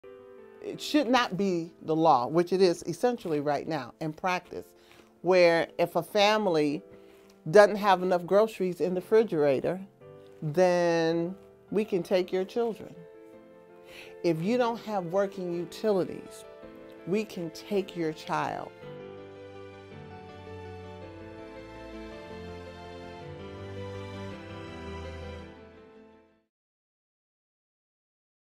Not Enough Groceries?
“It should not be the law—which it is essentially right now in practice—where, if a family doesn’t have enough groceries in the ‘frigerator, then we can take your children. If you don’t have working utilities, we can take your child.” - Attorney Allison Folmar, PRO Board Member